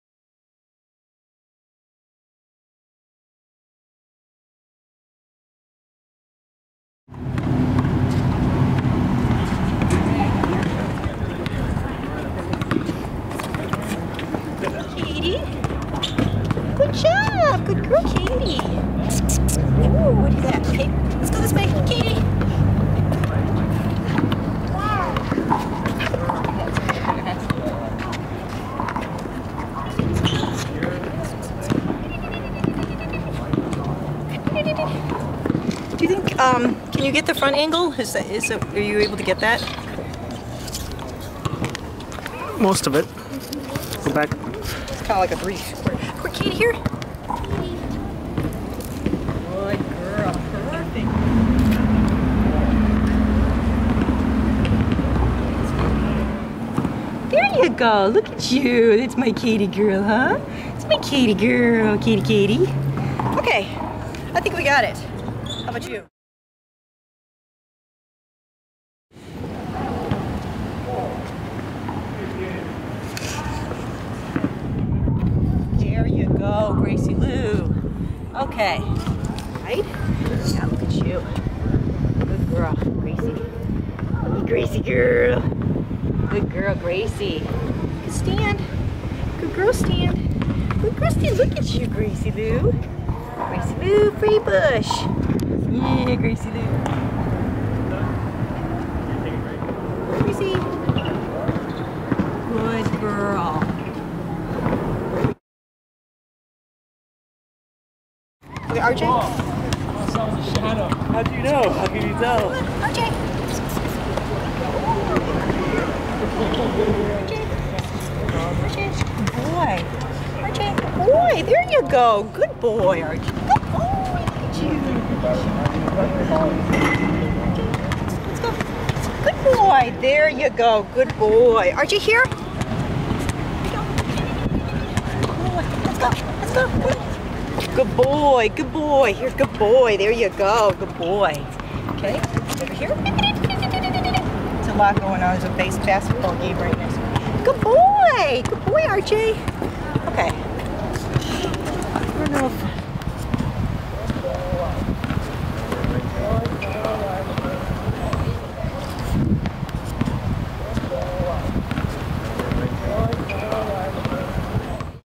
7 wk old Puppy Movement
This litter, together with my upcoming litter out of "OTCH Nars" (which is 3/4 the same pedigree as this litter) is our next generation of CH/OTCH dogs at Aratone Shelties.  BIS BISS GRCH Braeleen Foreign Intrigue "Brian" X Westar's Keepsake "Farah".  Cobred with Mark Hersman/Westar.  RJ ("Rafa Junior") is co-owned with Mary Heitsman. Come visit our dogs